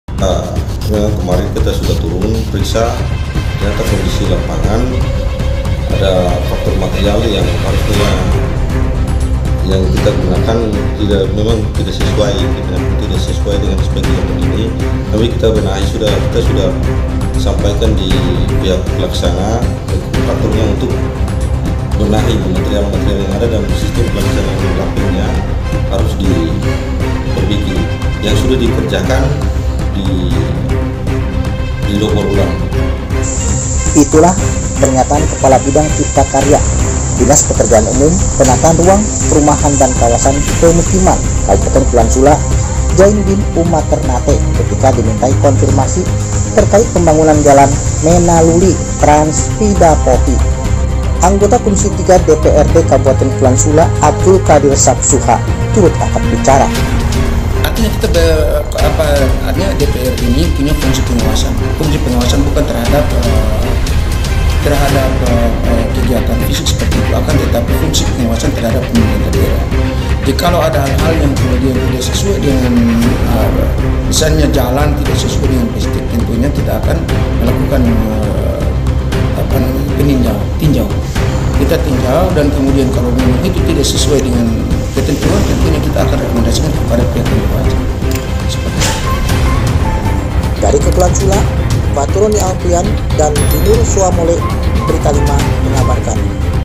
Pembangunan Jalan Menaluli-Trans Pidapohi Disoal
KEPULAUAN SULA, beritalima.com | "Kita sudah turun ternyata kondisi lapangan ada material yang digunakan kontraktor tidak sesuai spesifikasi", itulah pernyataan Kepala Bidang Cipta Karya Dinas Pekerjaan Umum, Penataan Ruang, Perumahan dan Kawasan Pemukiman (DPUPRPKP) Kabupaten Kepulauan Sula, Zainudin Umaternate, Senin (27/12/2931) ketika dimintai konfirmasi terkait pembangunan jalan Menaluli-Trans Pidapohi.

Proyek senilai Rp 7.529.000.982,00 sepanjang 4,4 kilometer menggunakan anggaran Dana Alokasi Khusus (DAK) 2021 itu diduga menyalahi Rencana Anggaran Biaya (RAB) dan material yang digunakan tidak sesuai spesifikasi.

Anggota Komisi III Dewan Perwakilan Rakyat Daerah (DPRD) Kabupaten Kepulauan Sula, Abdul Kadir Sapsuha turut angkat bicara terkait proyek yang dikerjakan PT HBM sesuai kontrak nomor 36/SPJ/PPK/BM/DPUPRPKP - KS/V/2021, tanggal 14 Mei 2021.      (Pathuroni Alprian dan Dinnur Soamole)